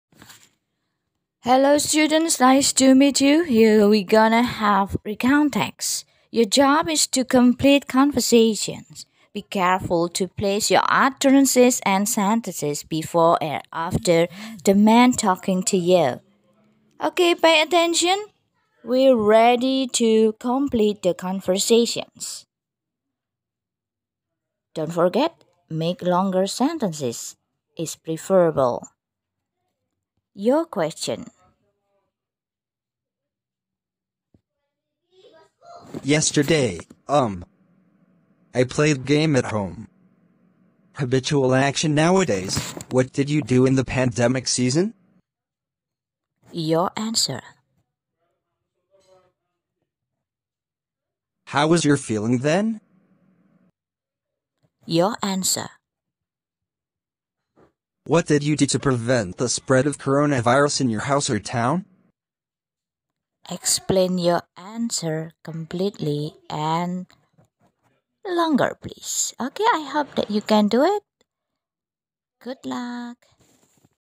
mengajar blended learning materi Recount text Day 9 fokus Completing Conversation
1. Watch the video

2. Write the questions (tulis pertanyaan atau jawaban orang laki laki di video)

3. Complete the text (lengkapi percakapan andaikan anda sebagai YOU pertanyaan dan jawaban apa yang cocok untuk dipercakapan agar nyambung dengan omongan laki laki di video)

4. Then record your voice or make video or write the text (pilih pilihan model tugas anda, mau anda buat video percakapan ulang antara laki laki dan kau, atau rekam suara jawaban anda, atau tulis teks saja, nilai ada dibawah ini)

Rubrik
Make video score maks 95 (berterima)
Make voice note maks 85 (berterima)
Make written text only maks 75 (berterima)